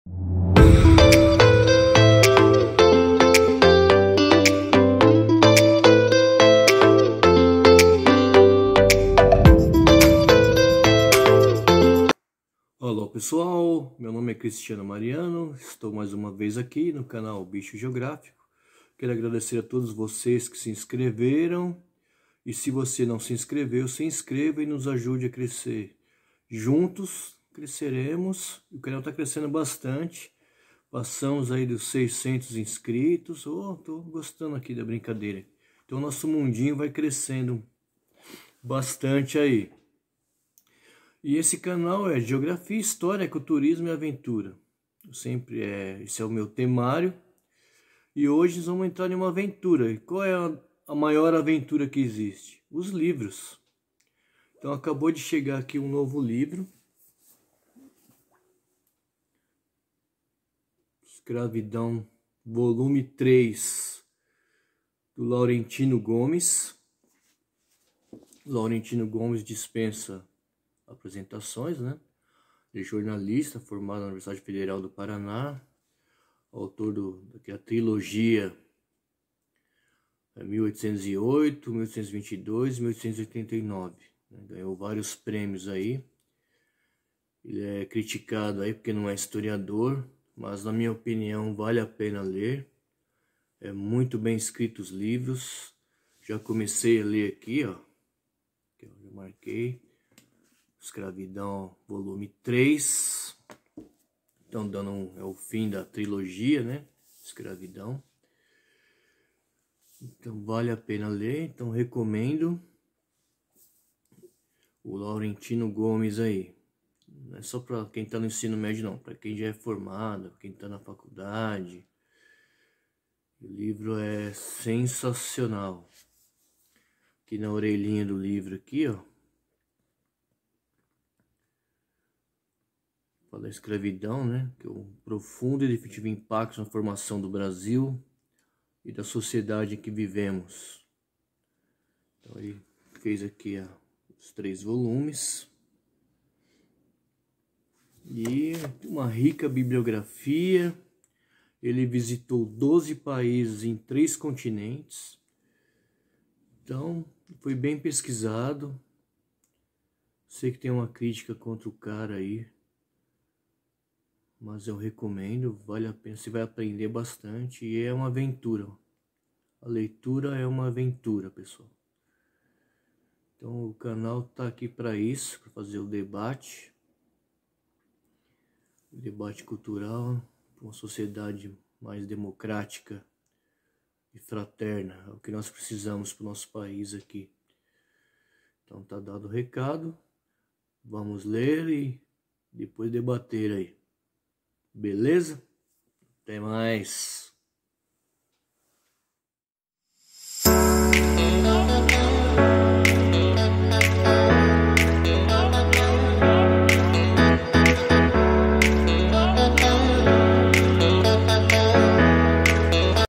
Escravidão volume3
Olá pessoal!
Eu sou Cristiano Mariano e este é o canal BiXo Geográfico!
Neste vídeo eu faço a apresentação do livro Escravidão Volume 3 de Laurentino Gomes, não estou ganhando nada pra fazer divulgação, aqui é simplesmente acadêmico, uma recomendação de uma boa leitura que fecha a trilogia. Laurentino é um ótimo autor, escreve  muito bem por isso indico para vocês bixoosgeográficos. Muito obrigado! VALEU!
Slavery volume 3!
Hello guys!
I'm Cristiano Mariano and this is the BiXo Geografico channel!
In this video I present the book Slavery Volume 3 by Laurentino Gomes, I'm not earning anything to promote it, here it's simply academic, a recommendation for a good read that closes the trilogy. Laurentino is a great author, he writes very well so I recommend it to you bixoosgeographics. Thank you very much! VALEU!
Esclavitud volumen 3!
¡Hola gente!
¡Soy Cristiano Mariano y este es el canal BiXo Geografico!
En este video les presento el libro Slavery Volumen 3 de Laurentino Gomes, no gano nada por promocionarlo, aquí es simplemente académico, una recomendación de buena lectura que cierra la trilogía. Laurentino es un gran autor, escribe muy bien por eso te lo recomiendo bixoosgeographics. ¡Muchas gracias! ¡VALEU!

                                                                  BIBLIOGRAFIA
GOMES, Laurentino. Escravidão: da independência do Brasil à Lei Áurea. Rio de Janeiro: Globo Livros, 2022.